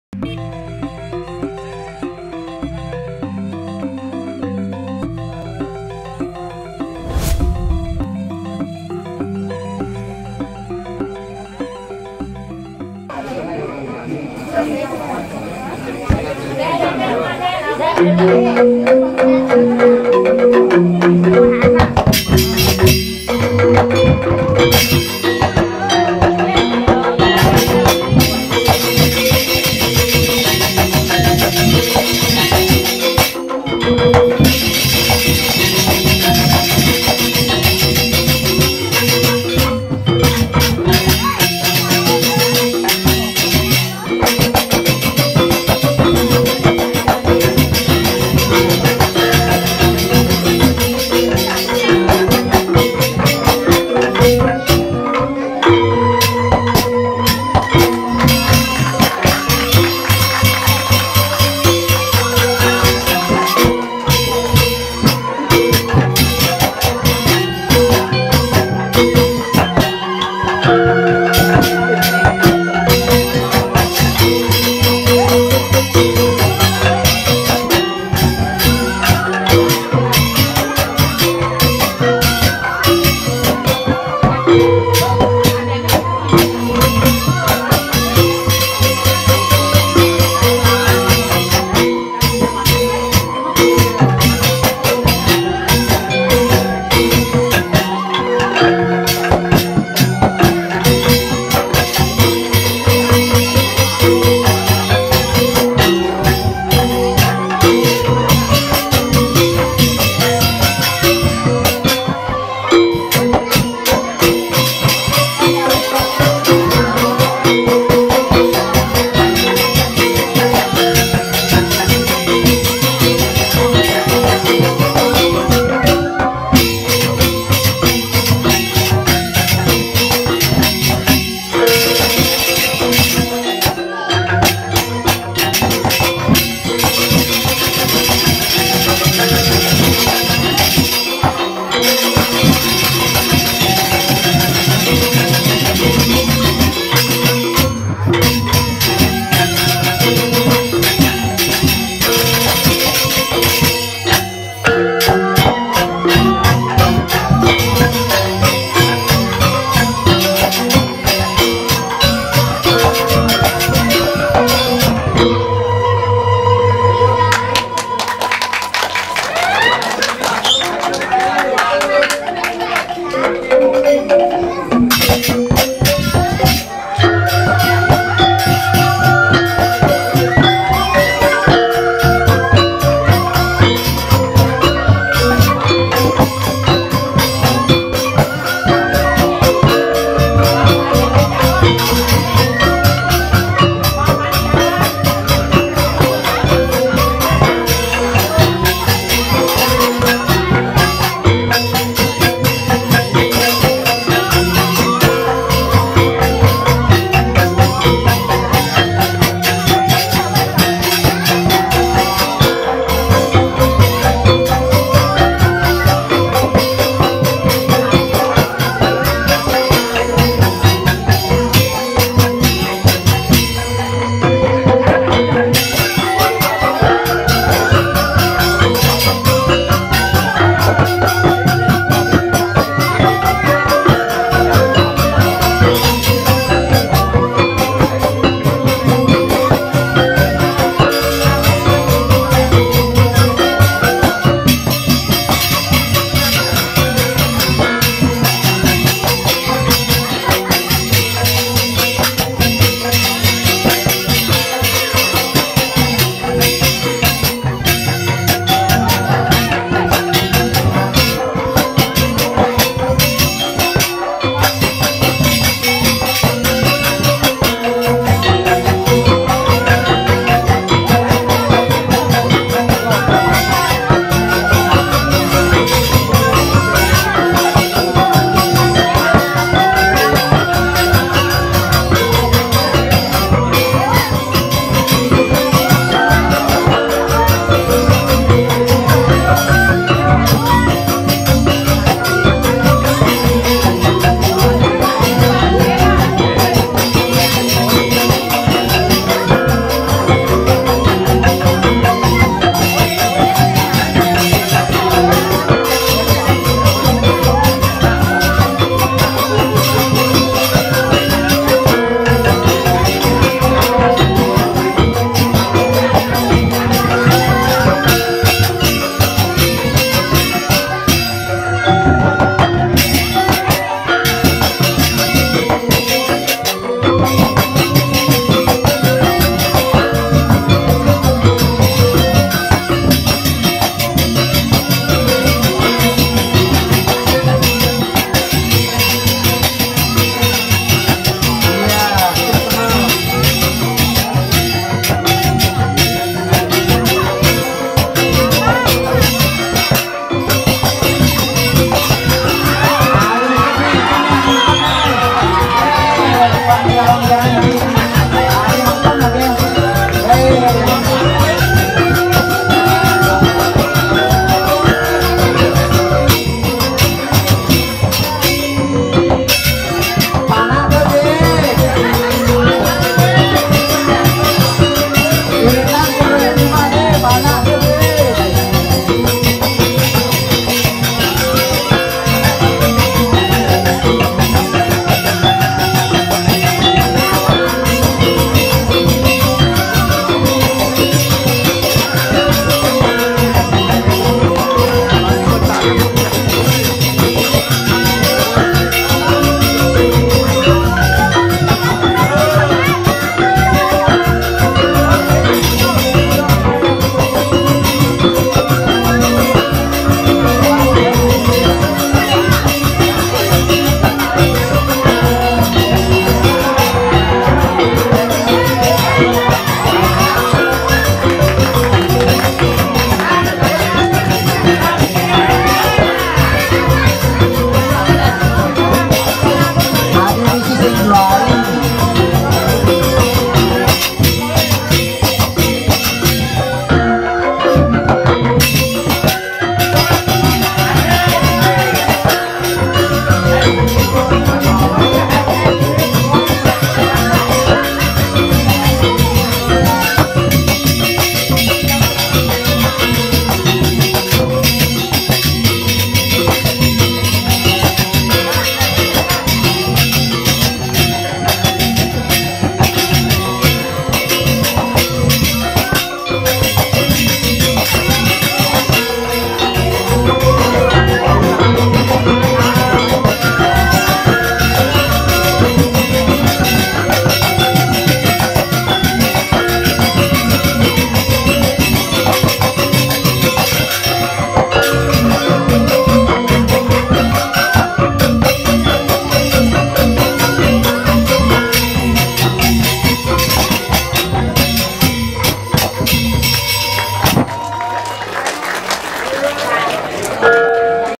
febriyani tampil cantik bersama sjk tari darma sulasih pengelatan
SKJ TARI DARMA SULASIH DESA PAKRAMAN PENGELATAN
Penari : Febriyani